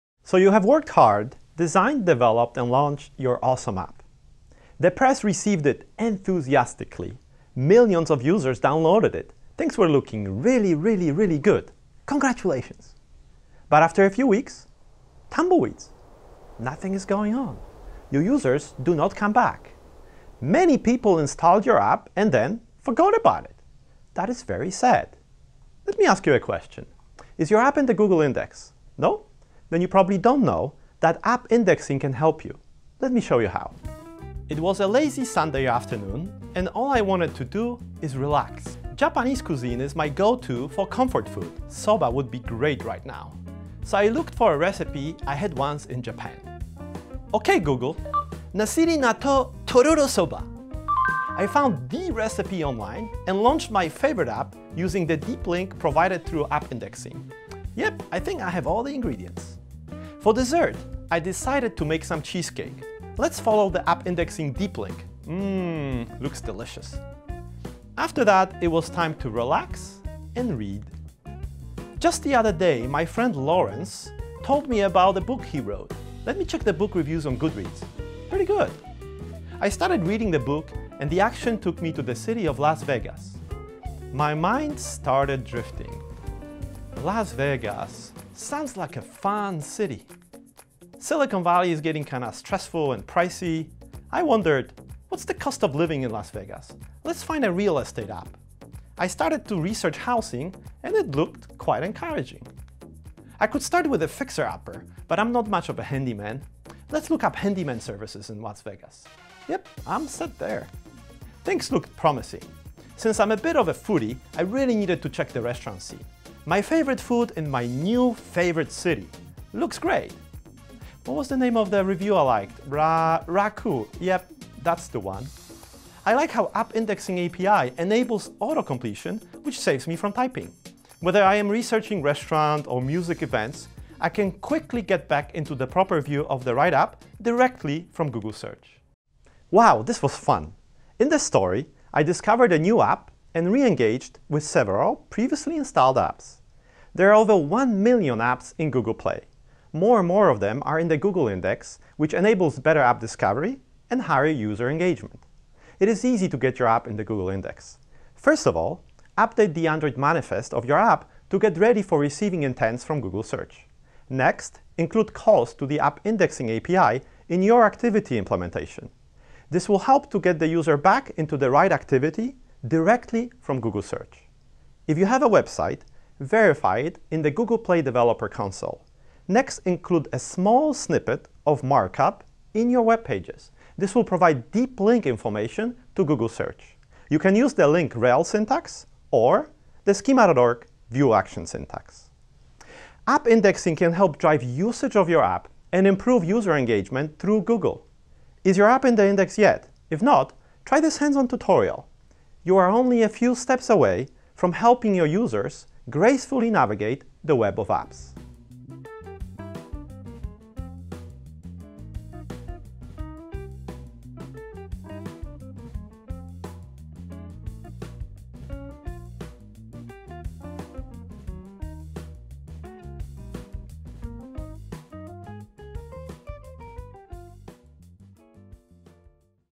Is your app in the Google index? DEPRECATED
Get your app in the Google index — and be ready for the future of search!

In 1998, Google set out to index the web and make content easily accessible and discoverable. Today mobile app adoption is growing rapidly and we’re bringing the same accessibility and discoverability to apps. App Indexing is the ability for Google to index apps just like websites. Deep links to your Android app appear in Google Search results so users can get to your native mobile experience quickly, landing exactly on the right content within the app.

Drive return visits to your app

￼￼For users who have your app installed, having deep links appear in search results will help drive organic traffic back to your app.

Re-engage your users

For users who have viewed pages in your app, and later searches for similar content, you can use the App Indexing API to have deep links appear in search suggestions. For example, a user visits a pizza restaurant page in an app. The next time the user searches for pizza, a deep link will appear in search suggestions before seeing search results.

Create a seamless experience for your users

App indexing is flexible—you can direct search users to your app or website on a page by page basis. Moving from search results to apps is seamless, without any pop-ups or extra taps to slow users down.